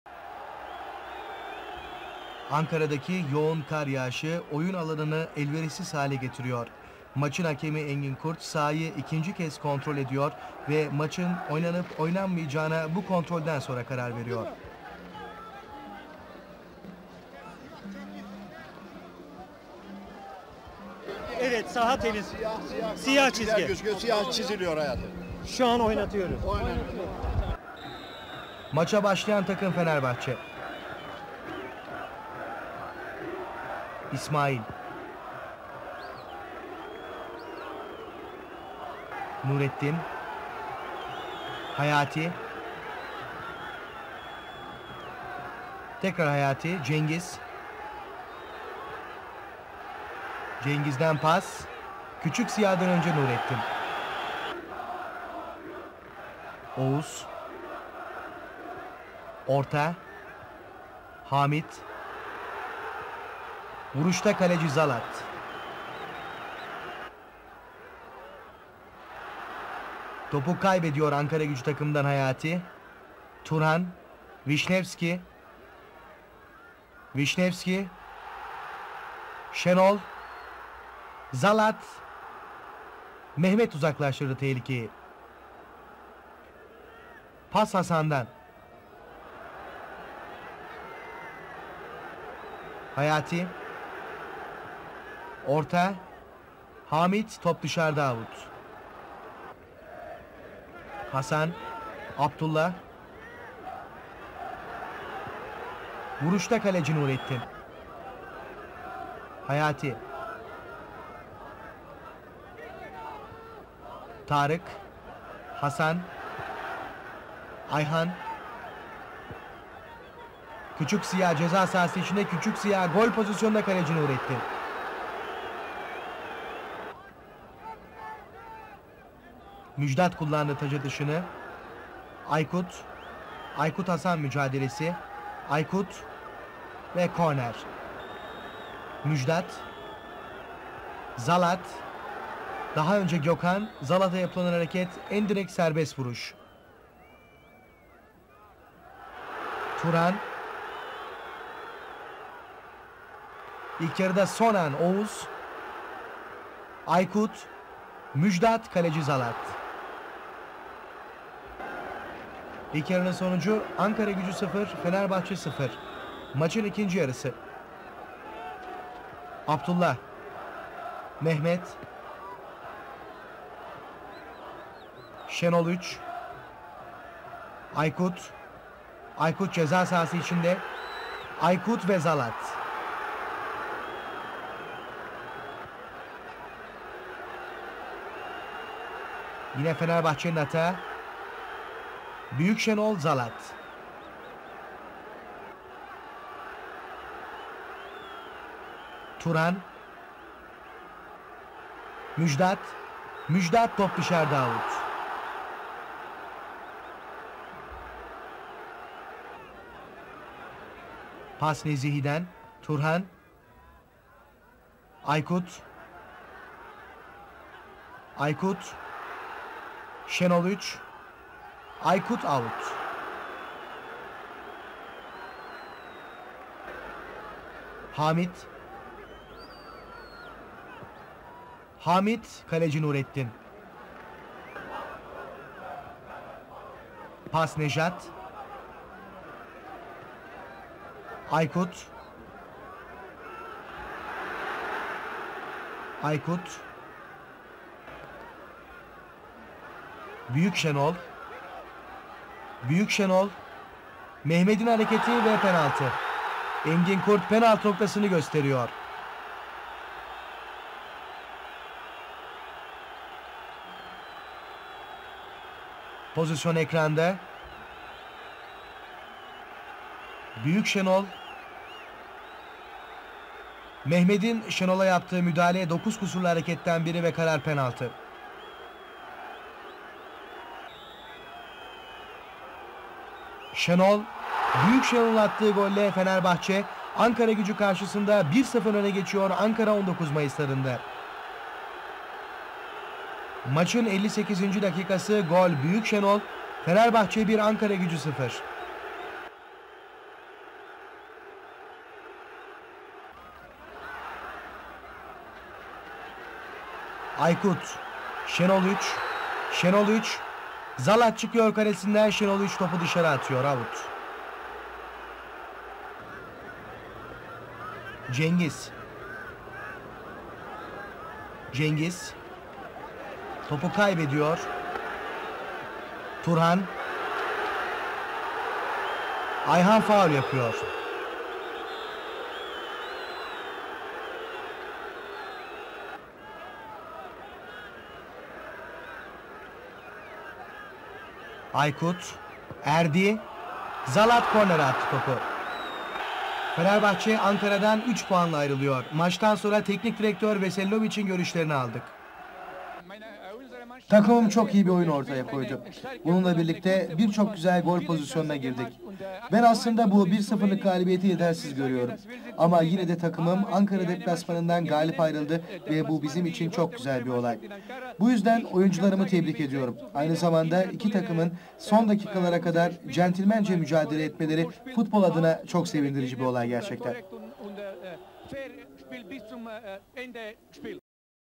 1989 1990 Ankaragücü Fenerbahçe BU ZEMİNDE BU KADAR 15.Hafta Maçı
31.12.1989
ankaragücü: rade zalad, mehmet soykök (dk. 80 yaşar altıntaş), hamit yüksel, gökhan gedikali, abdullah duran, ayhan akyüz, cengiz alp, hayati soydaş, hasan sönmez, tarık üstün, ziya yıldız
teknik direktör: necdet niş
fenerbahçe: nurettin yıldız, müjdat yetkiner (dk. 78 erdi demir), necat barut, turhan sofuoğlu, oğuz çetin (dk. 88 vedat uysal), şenol ulusavaş, ismail kartal, ıvan vichnevski, nezihi tosuncuk, şenol çorlu, aykut kocaman
teknik direktör: todor veselinovic (yugoslavya)
gol:
(0-1) şenol çorlu dk. 58 (pen.)

sarı kart:
ayhan akyüz dk. 61 (ankaragücü)